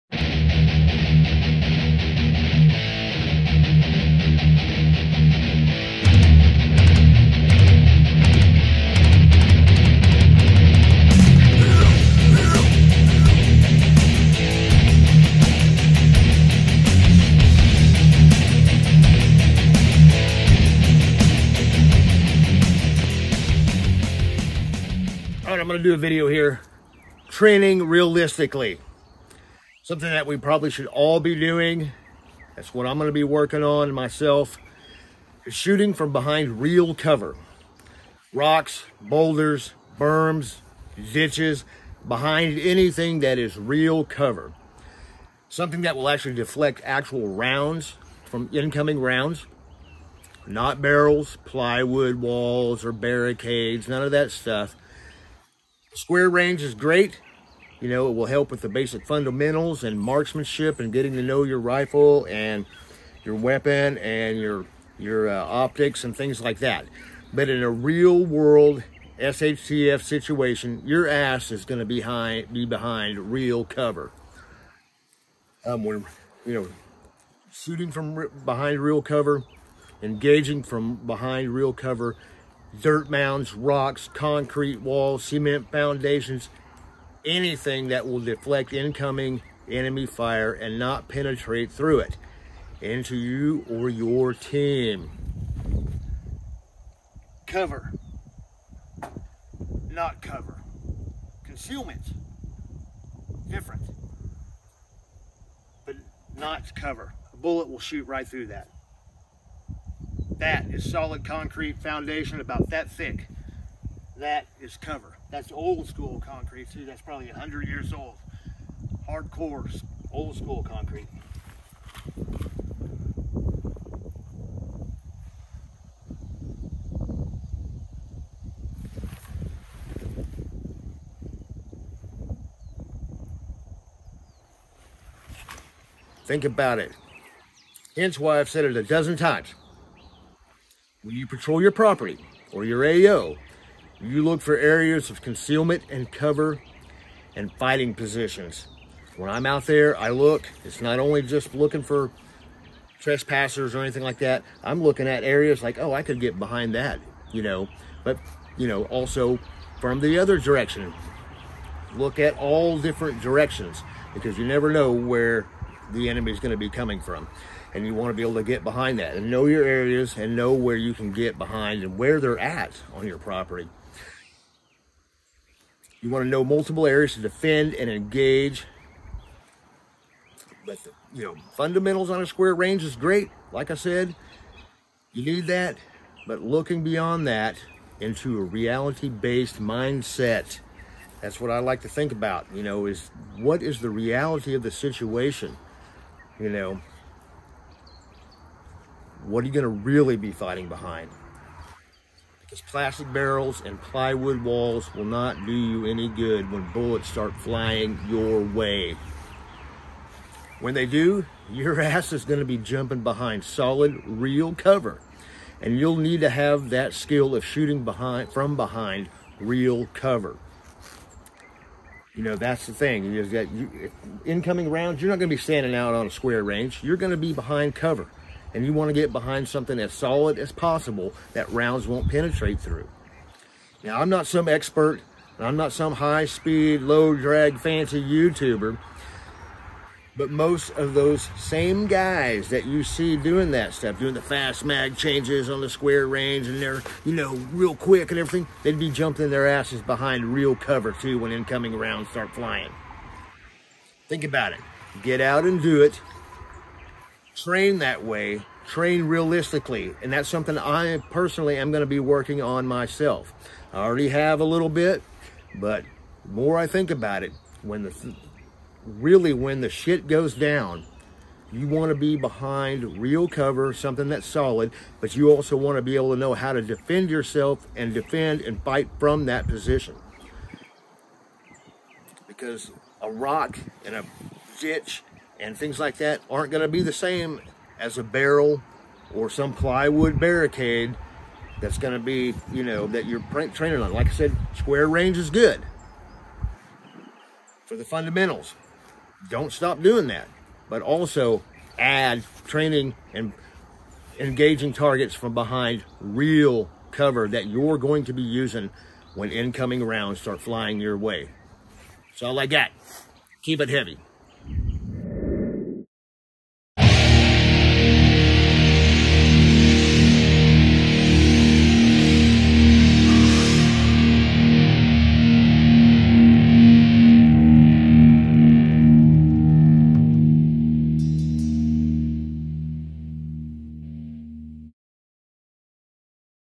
TRAIN REALISTICALLY!
Square Range is great for the fundamentals. But train like how your really going to engage and defend from, from behind real cover.